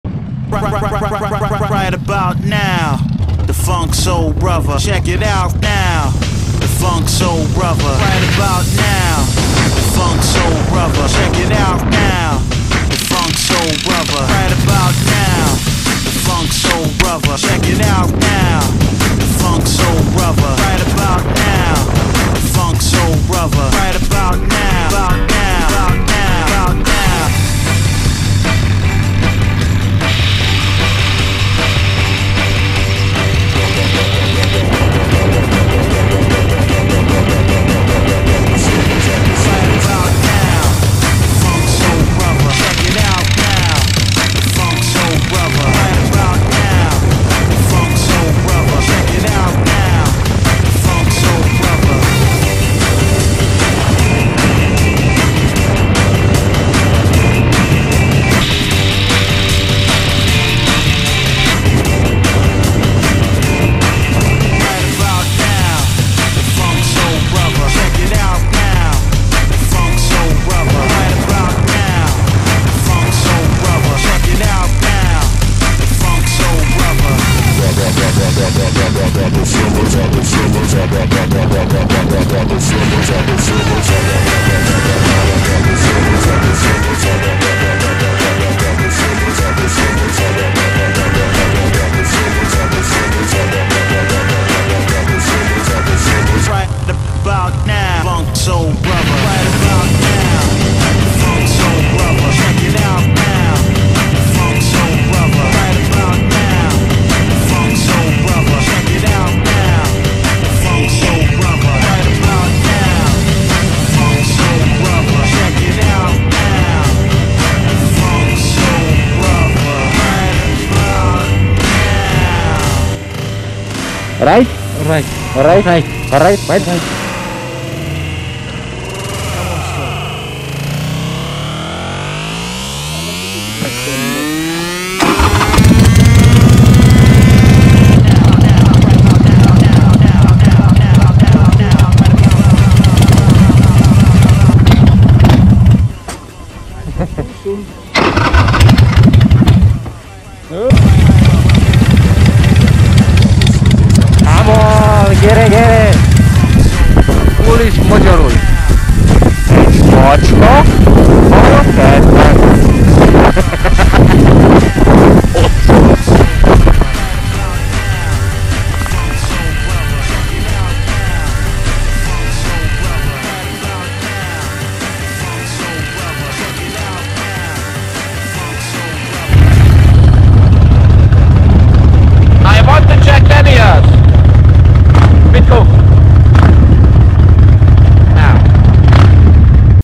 Szczur and Mamba around the Rock
Two friends riding their Harleys around the island of Jersey.